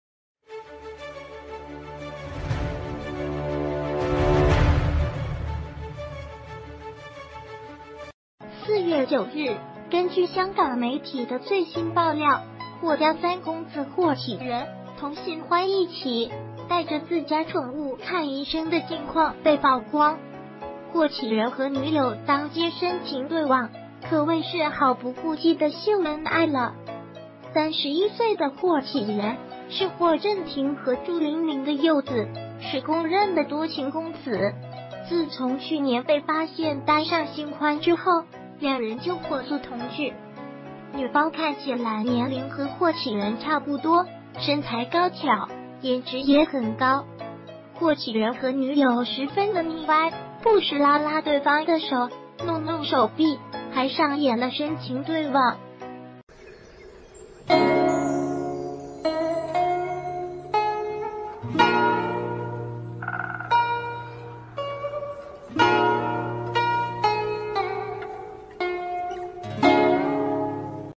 霍啟剛三弟霍啟仁罕見露面 與性感女友當街親昵
4月9日，根據香港媒...